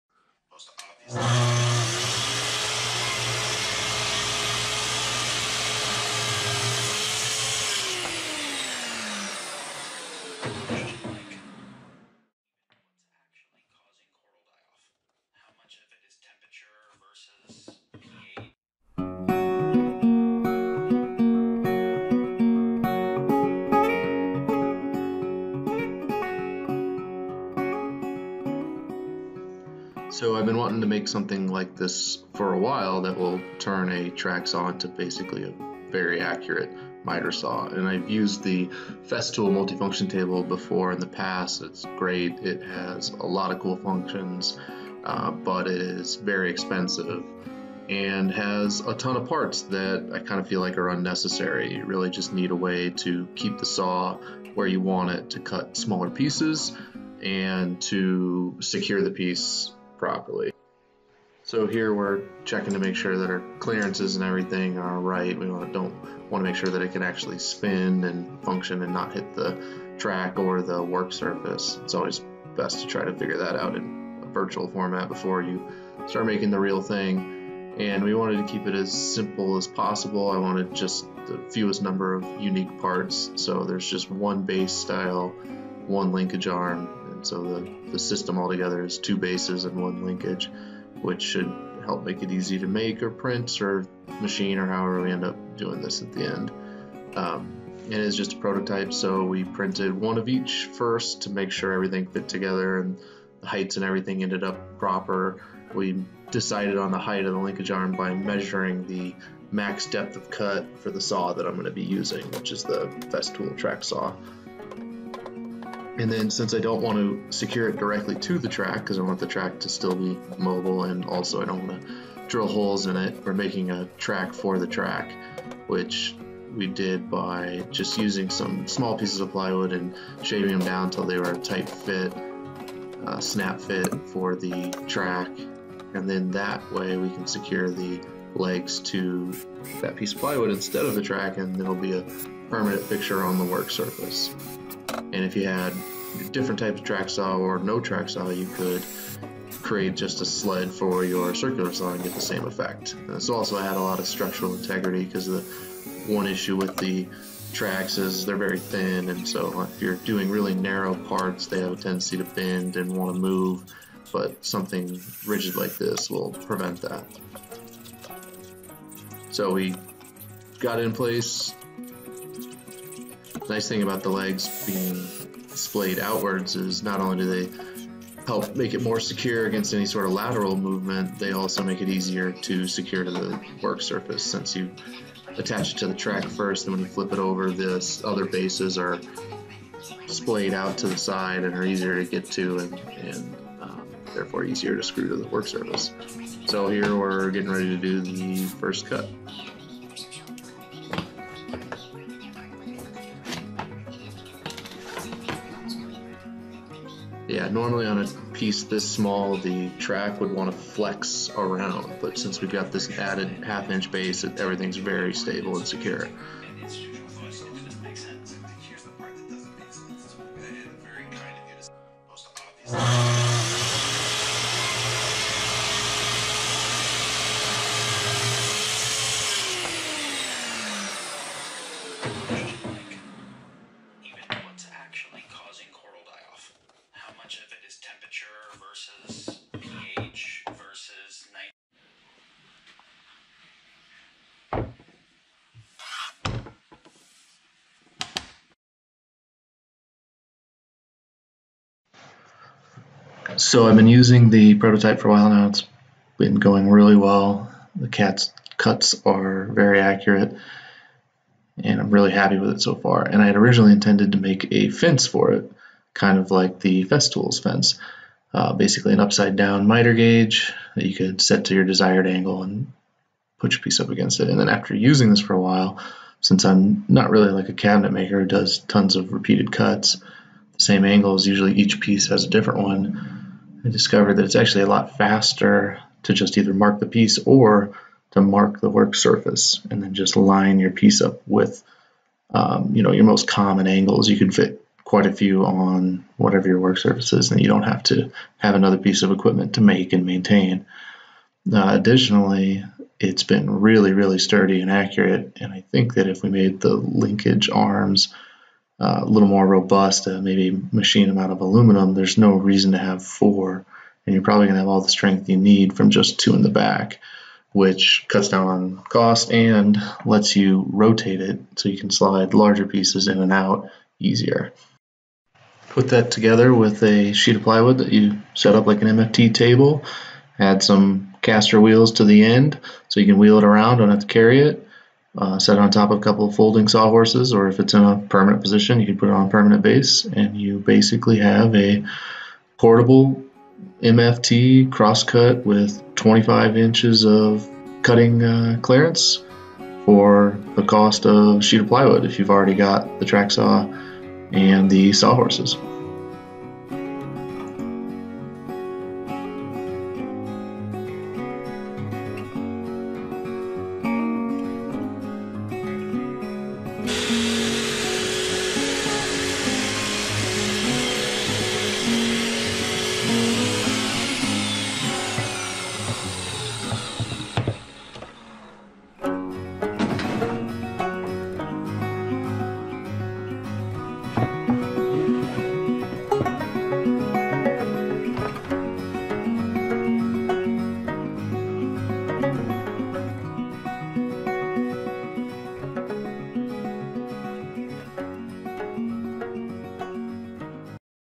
Track Saw Cross Cut Lift || 3D Printer || raaz design
Cutting small or narrow pieces has always been a challenge for the track saw and since it's my preferred tool of choice I made these parallel lifts to extend it's usability. In effect I think it turns the track saw into a miter saw with a great cutting capacity and without having to buy another tool or dedicate a bunch of space to a miter station.

I plan on carrying this design forward and incorporating it into a mobile workstation to take to job sites.